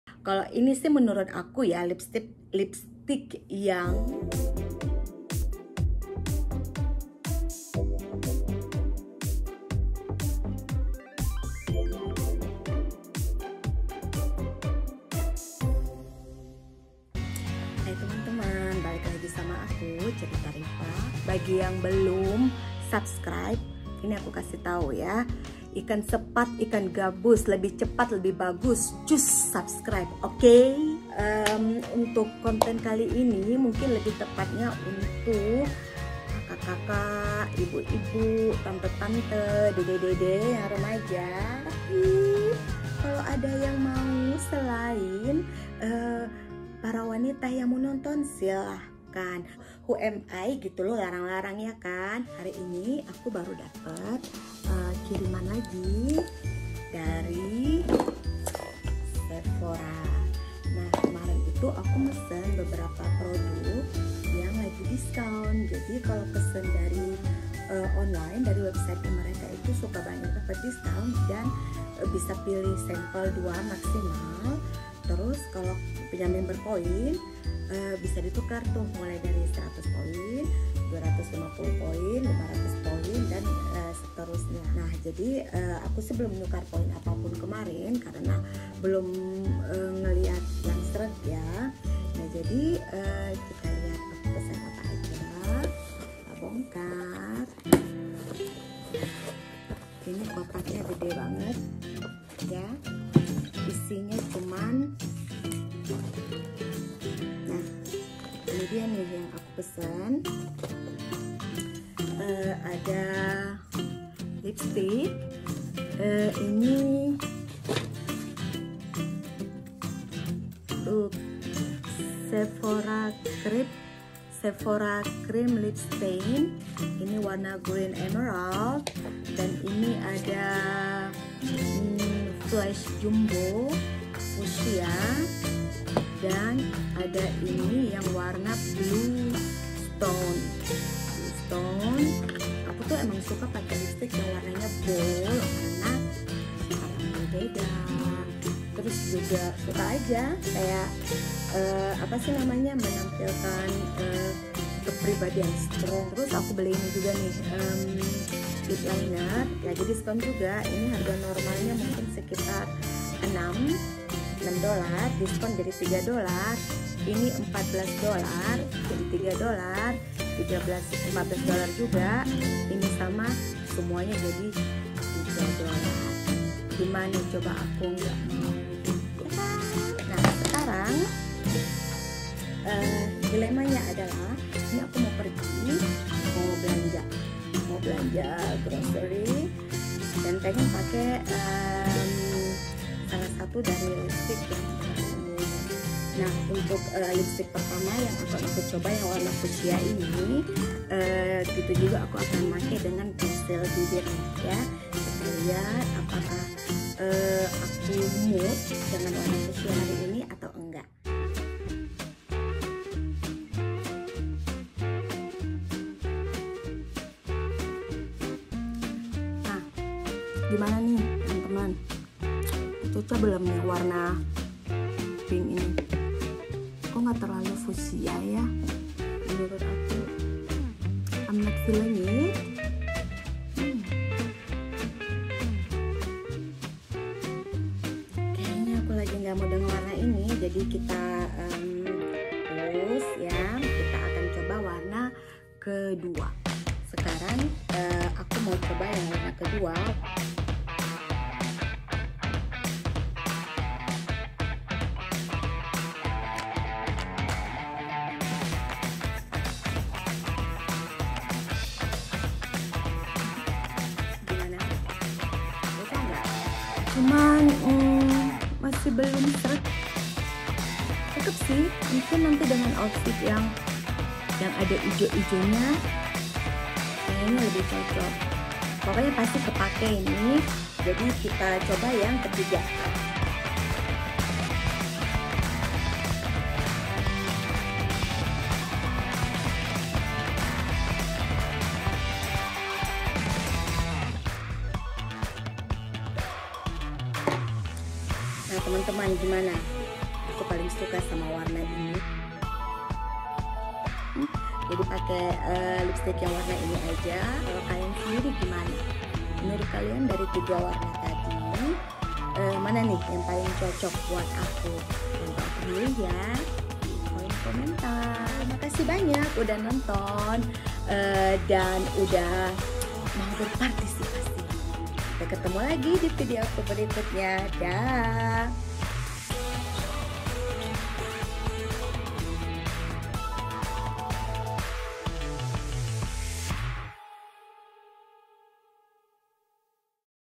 UNBOXING DAN REVIEW LIPSTIK DISKON DARI SEPHORA II ENGLISH SUBTITLED
Hi semua!

Setelah dicoba coba, aku suka semuanya. Menurut kalian aku paling cocok pake warna apa ni?
Yuk kasih tau aku di kolom komentar ya.

Terimakasih.

Love,
Riffa